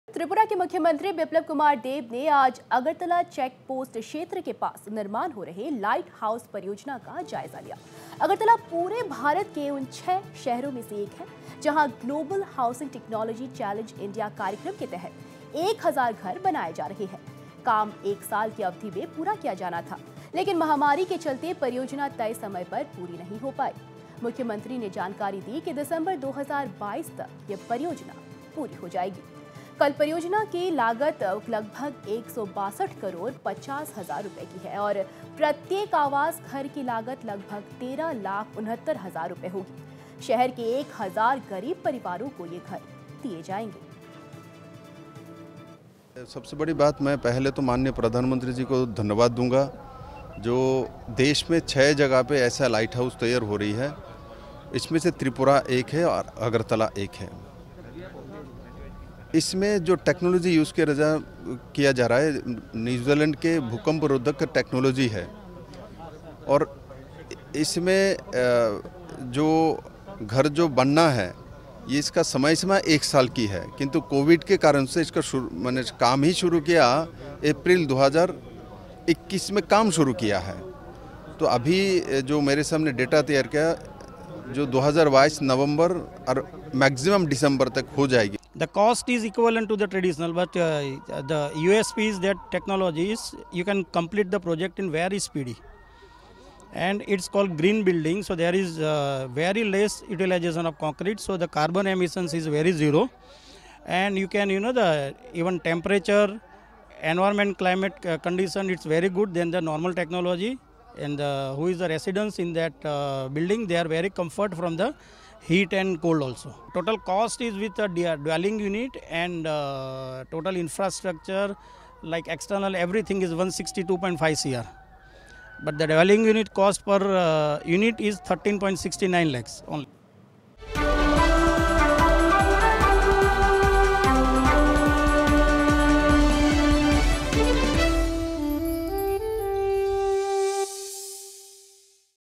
लाइट हाउस परियोजना का जायजा, 1,000 घर बनाए जा रहे
Tripura | त्रिपुरा के मुख्यमंत्री बिप्लब कुमार देब ने आज अगरतला में निर्माण हो रहे लाइट हाउस परियोजना का जायजा लिया. अगरतला पूरे भारत के उन छह शहरों में से एक है जहां ग्लोबल हाउसिंग टेक्नोलॉजी चैलेंज-इंडिया कार्यक्रम के तहत 1,000 घर बनाए जा रहे हैं। मुख्यमंत्री ने जानकारी दी कि दिसंबर 2022 तक यह परियोजना पूरी हो जायेगी. शहर के 1000 गरीब परिवारों को यह घर दिए जाएंगे.